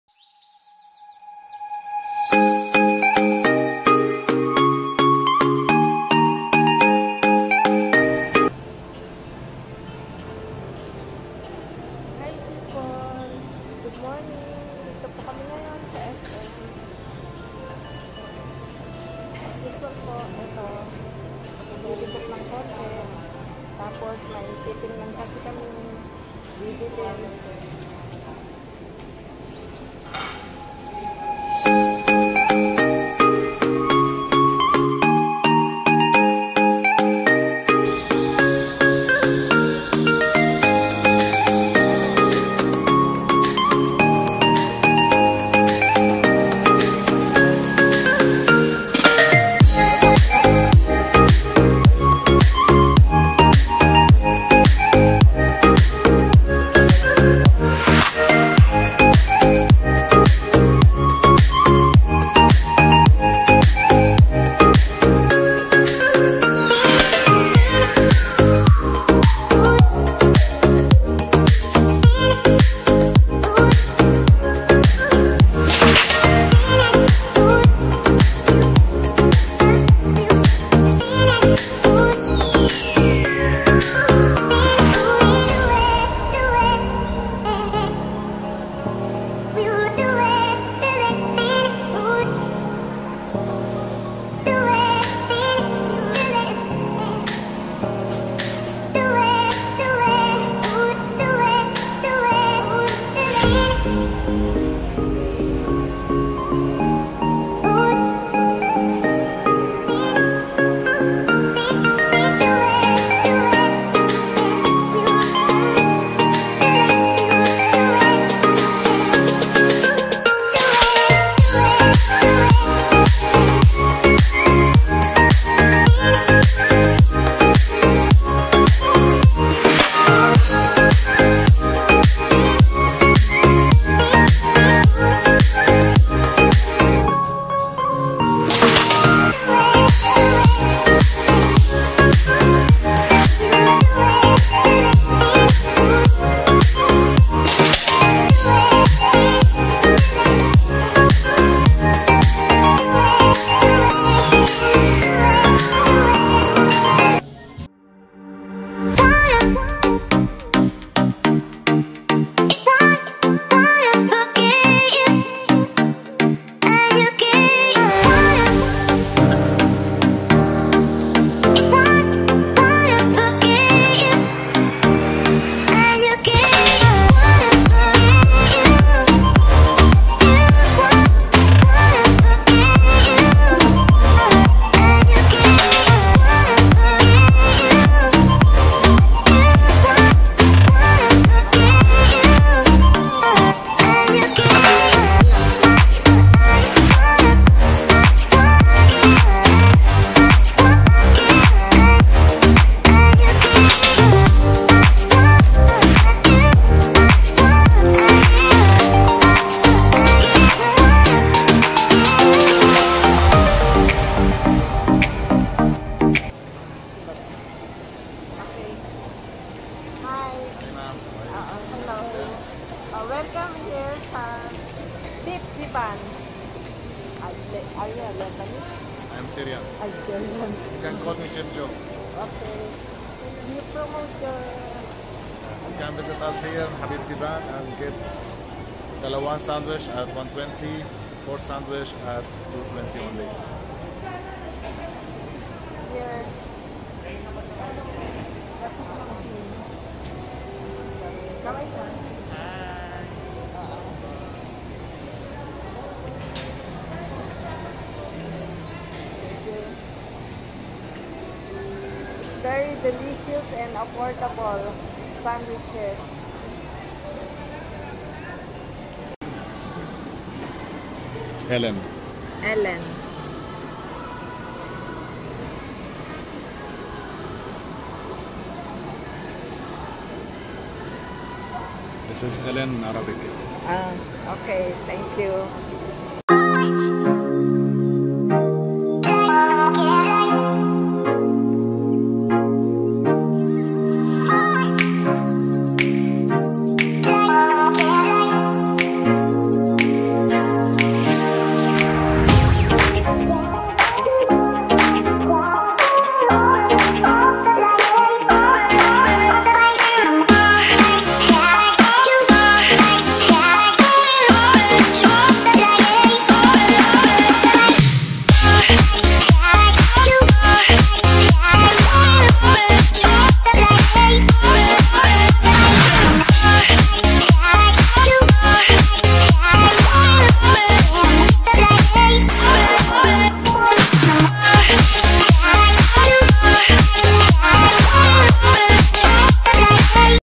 FIRST TIME IN SM DASMARINAS
SM Dasmarinas

Habibti Bun

Our first time in SM Dasmarinas and it"s far from our place.. We went there to look & buy that gift for our grandchild.. Thanks that we able to find one & also met special people out there. Have a nice day everyone.